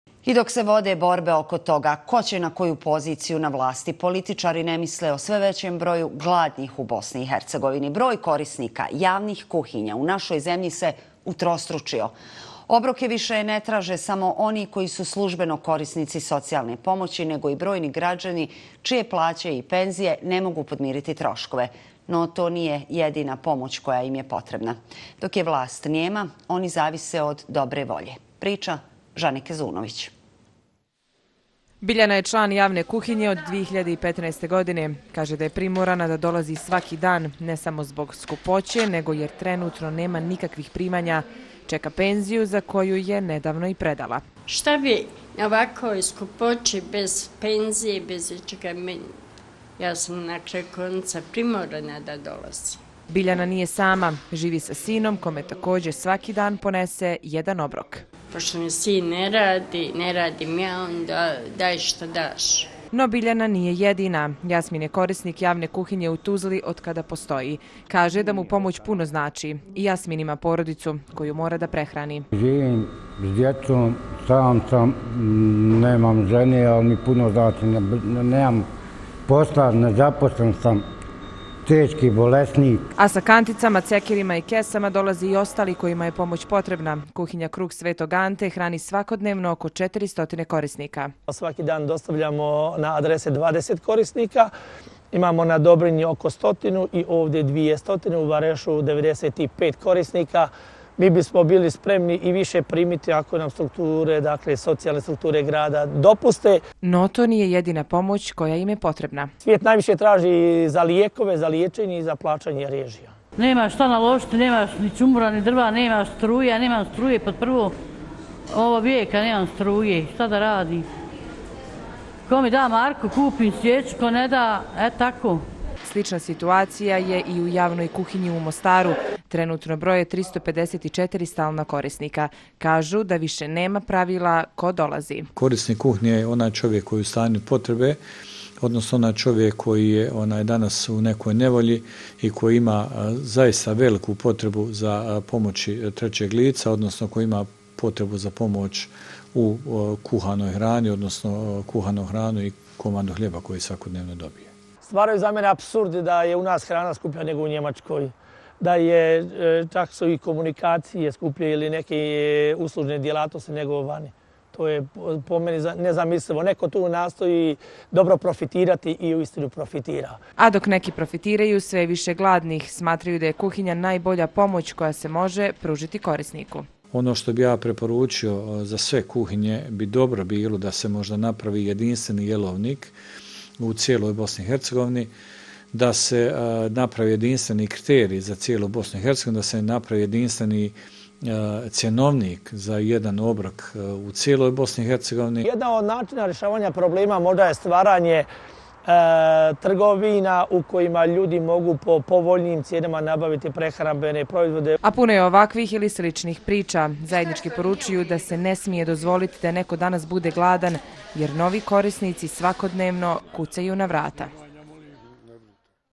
Sve duži redovi pred javnim kuhinjama, dok je vlast nijema, oni zavise od dobre volje
Dok se vode borbe oko toga ko će na koju poziciju na vlasti, političari ne misle o sve većem broju gladnih u BiH. Broj korisnika javnih kuhinja u našoj zemlji se utrostručio. Obroke više ne traže samo oni koji su službeno korisnici socijalne pomoći, nego i brojni građani čije plate i penzije ne mogu podmirit troškove. No, to nije jedina pomoć koja im je potrebna. Dok je vlast nijema, oni zavise od dobre volje.